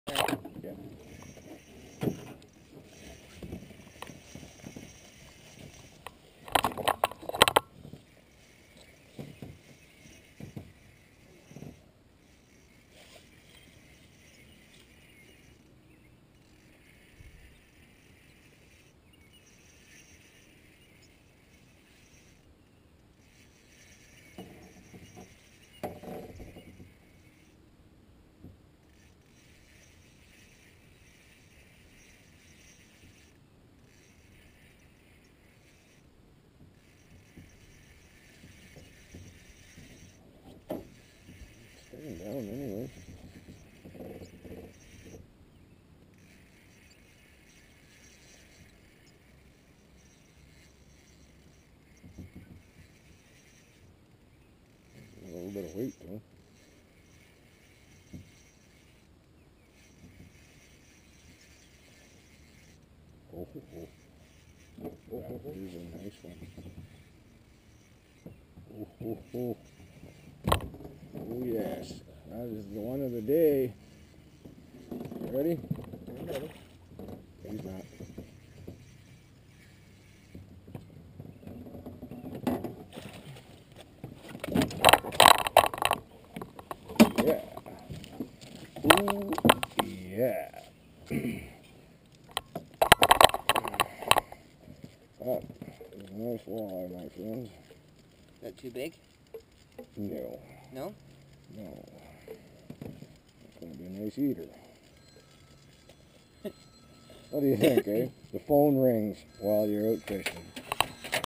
Pickerel for lunch
Nothing is better than fresh pickerel for lunch. Looks like we will be having a nice feast.